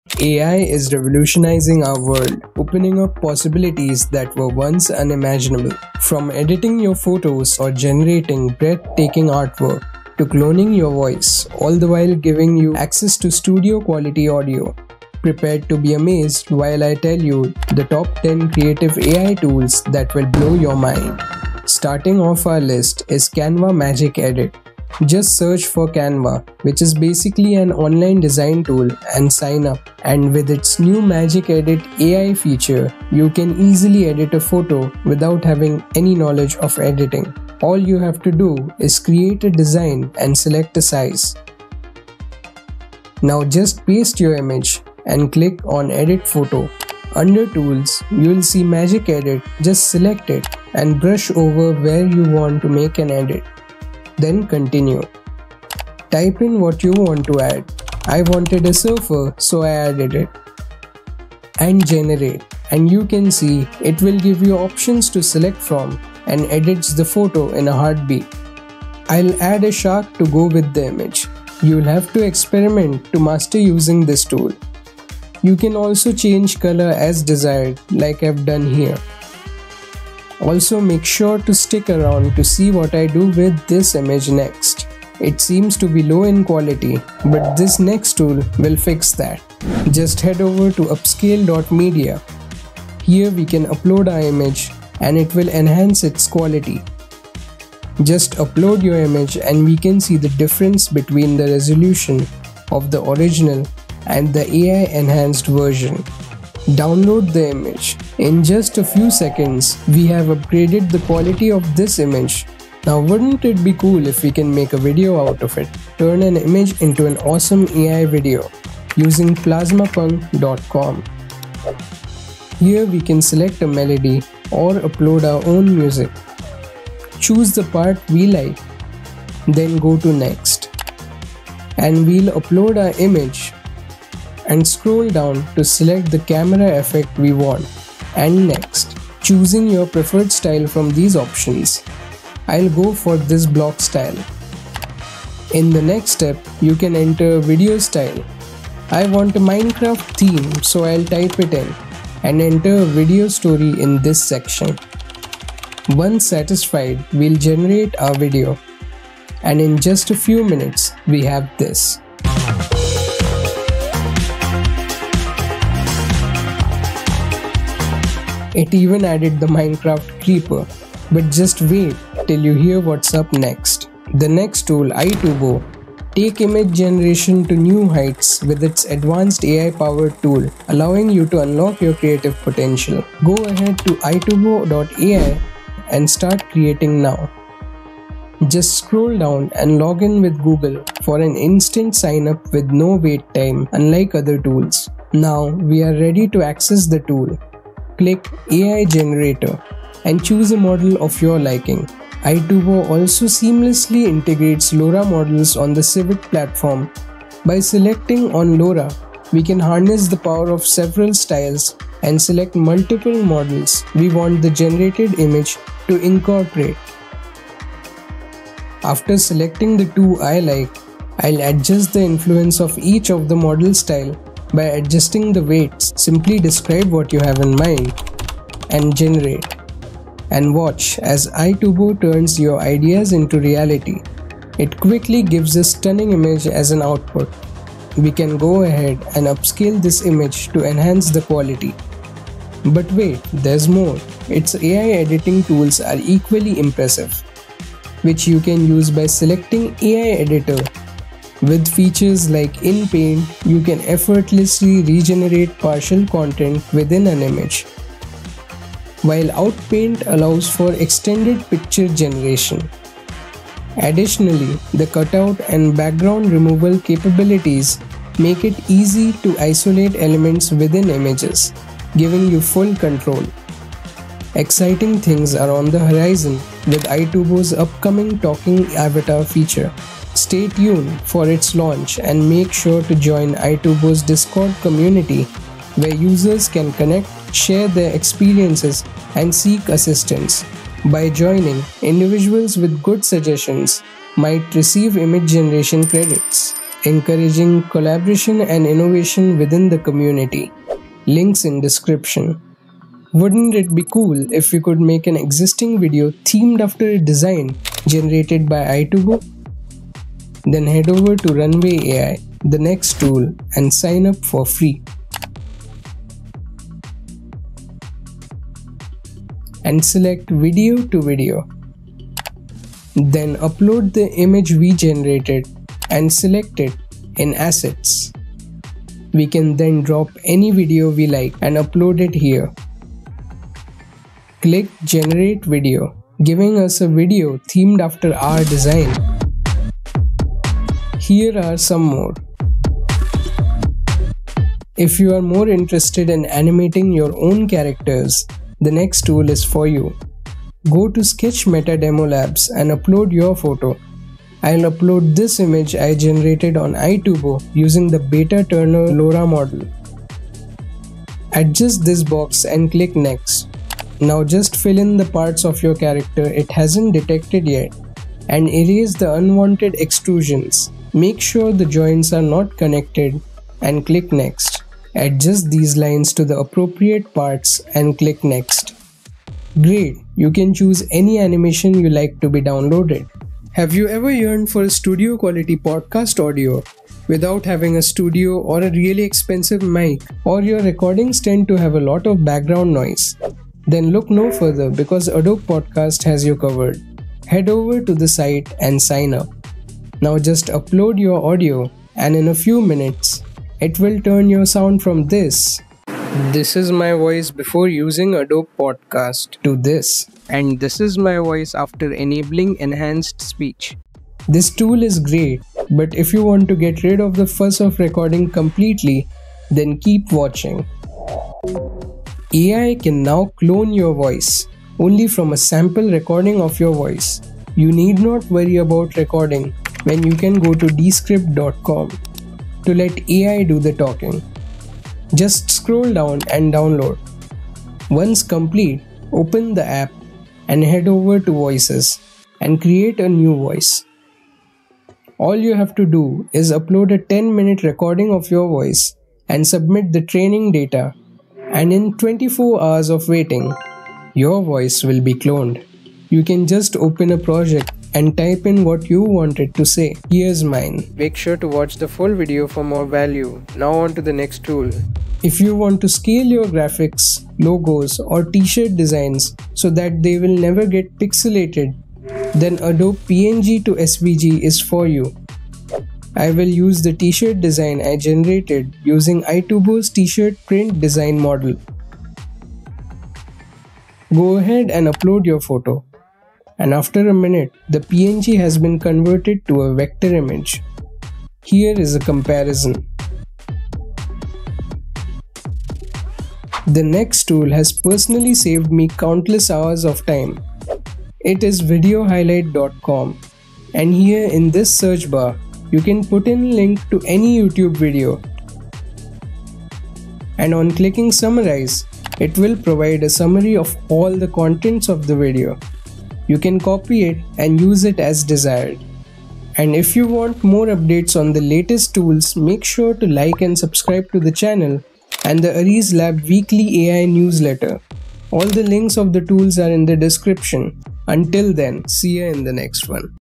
I Found The TOP 10 Creative AI Tools No One Talks About! (Don't Miss 🤯)
In this mind-blowing video, we unveil the top 10 Creative AI Tools that will revolutionize your creative journey, and the best part? They're all FREE! 🚀
Discover the capabilities of each tool as we explore their potential from designing stunning visuals to enhancing media, leaving you in awe.